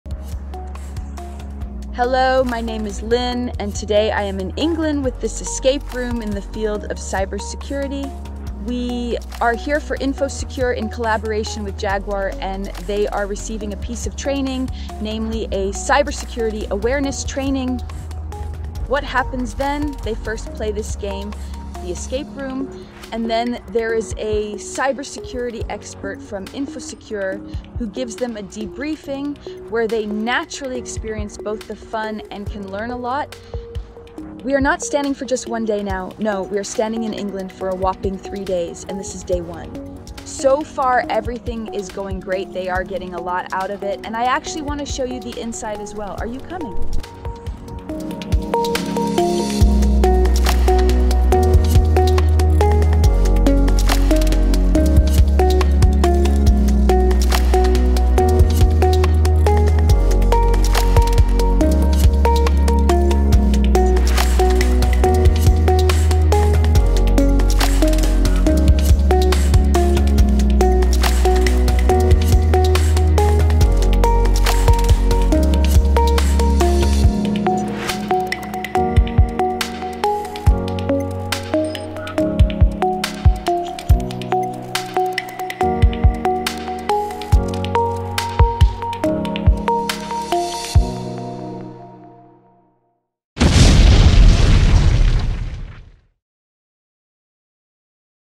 CYBER SECURITY ESCAPE ROOM AT YOUR COMPANY?
Do your colleagues still click on phishing emails or did you recently send out another pen test and more than half opened this email? One such click can cost your organization 4 percent in turnover. It's not a matter of if, but when you will be hacked. Man is the weakest link, but he can still change his behavior. Every behavioral change starts with awareness. You can create awareness, for example, through an impactful and fun training.

At Escape Room Designer we use escape rooms for training. Our Cyber ​​Security training is ideal for all your employees to take the first step towards a safer working environment. In this way, a change in behavior can take place. After this Awareness training, people are more likely to report a suspicious situation. In addition to a higher learning effect, you also have a great team day. This will be discussed for a long time to come.